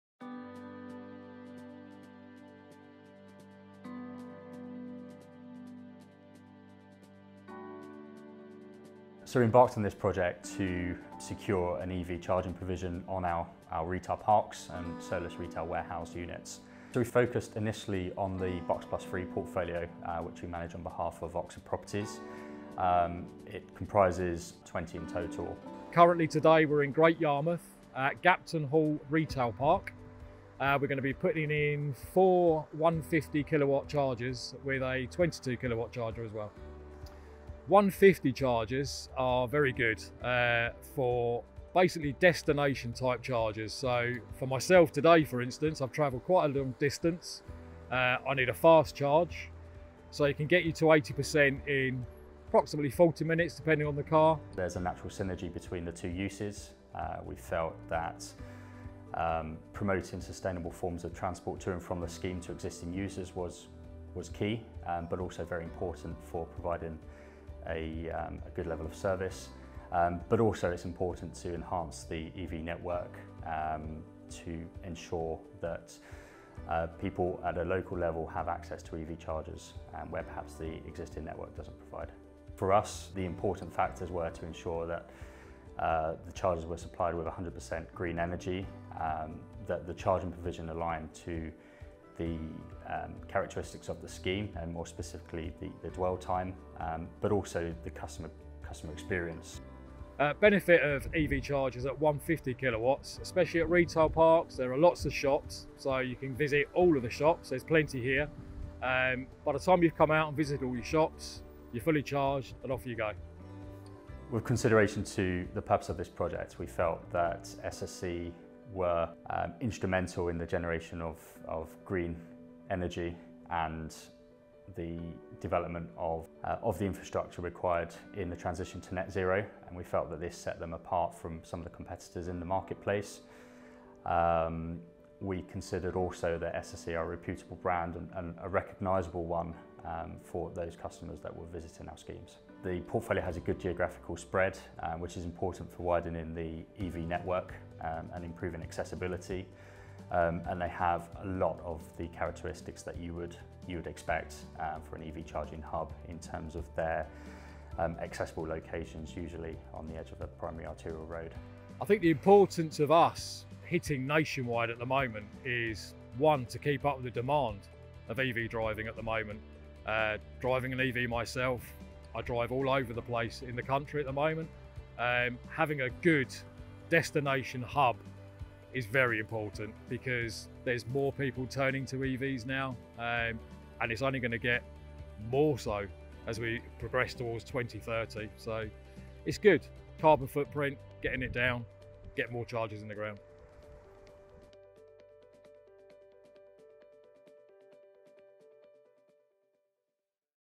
SSE Energy Solutions & M7 Real Estate - Phase 1 EV Charging Hubs on Retail Sites
Earlier this week we shared the news that work had begun on several of our EV Charging Hubs at retail parks across the country, in Phase 1 of a project between ourselves and M7 Real Estate.

We spoke with Asset Manager at M7, Josh Tyler, to find out more about their sustainability goals, why they chose us to deliver this work and what the positive benefits are for this roll out. We also visited one of the sites at Gapton Hall Retail Park in Great Yarmouth to catch up with our Project Delivery Manager, Jason Slater, where work was well underway.